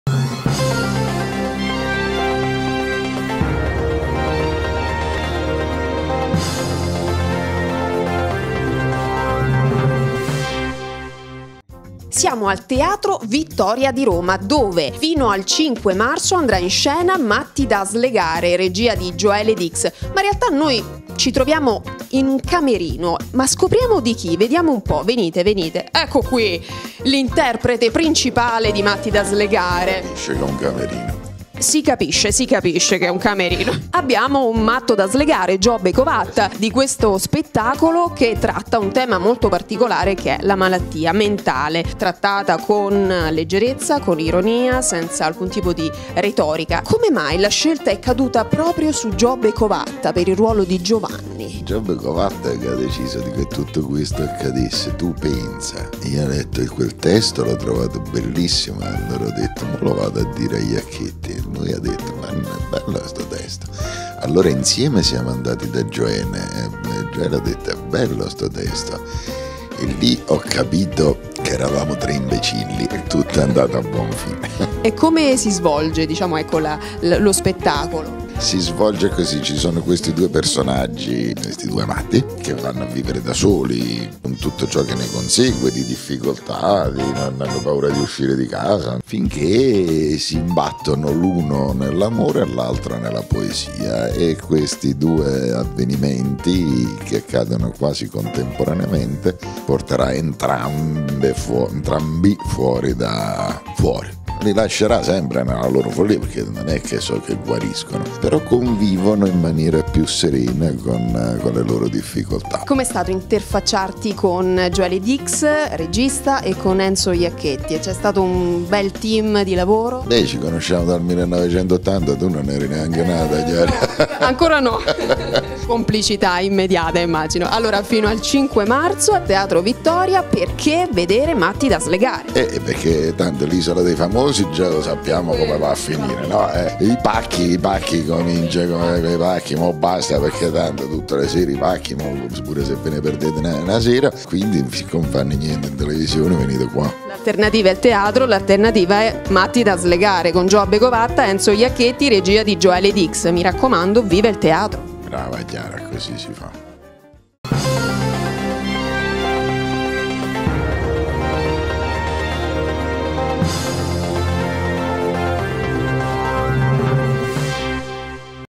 Matti da slegare.Di Gioele Dix con Enzo Iacchetti e Giobbe Covatta.Il servizio di Chiara Trabolotti
La commedia ironica che tratta il tema della malattia mentale, è in scena al Teatro Vittoria fino al 5 marzo.
Con il duo comico Enzo Iacchetti e Giobbe Covatta.
Il servizio per Canale 21 di Chiara Trabolotti